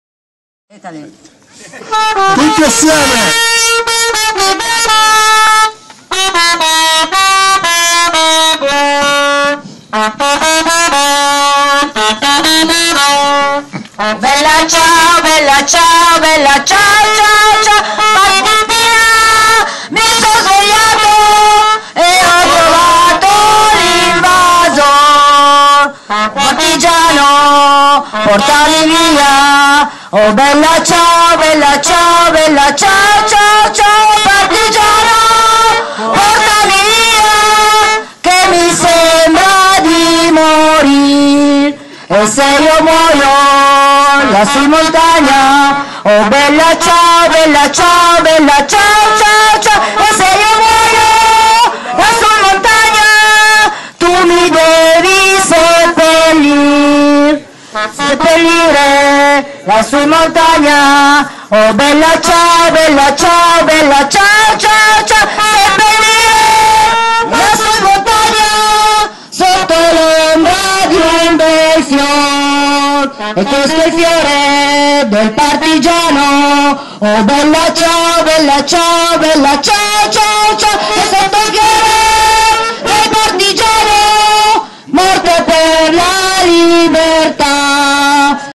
10-02-2013 Mattie - Fiaccolata di solidarietà ai due notav arrestati a Giaglione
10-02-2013 Mattie - Fiaccolata di solidarietà ai due notav arrestati a Giaglione